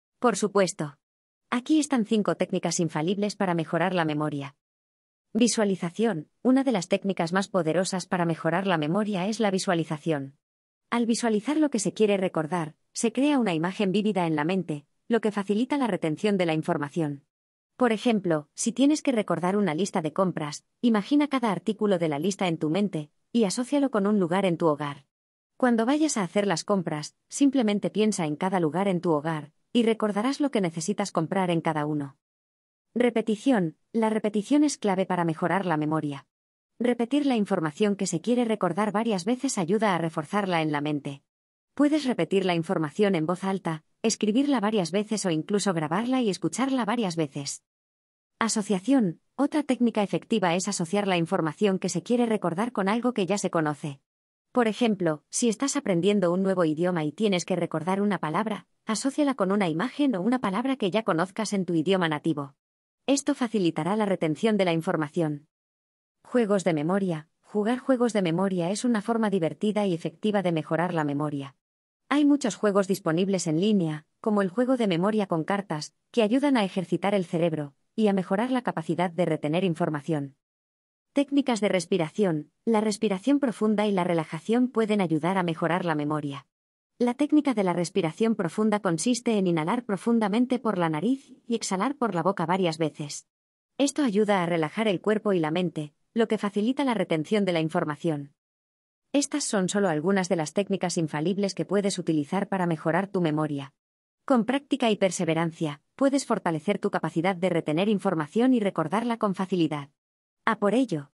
¡No vuelvas a olvidarte de nada! Las 5 técnicas que te garantizan una mejor memoria
¿Te gustaría recordar todo lo que aprendes, lees o escuchas? ¿Quieres tener una memoria prodigiosa y sorprender a tus amigos y familiares? Entonces, sigue leyendo y descubre las 5 técnicas infalibles para mejorar la memoria que te harán decir ¡nunca más olvides nada! 🙌

Técnica 1: Repite en voz alta lo que quieres memorizar. 🔊
Técnica 2: Asocia imágenes mentales con la información que quieres recordar. 🖼️
Técnica 3: Agrupa la información en categorías o listas. 📋
Técnica 4: Usa reglas mnemotécnicas o acrónimos. 🧠
Técnica 5: Repasa periódicamente lo que has aprendido. 📚

Estas 5 técnicas son fáciles de aplicar y te ayudarán a mejorar tu memoria de forma significativa. Pruébalas y verás cómo tu capacidad de recordar aumenta día a día. ¡No esperes más y empieza a mejorar tu memoria hoy mismo! 😊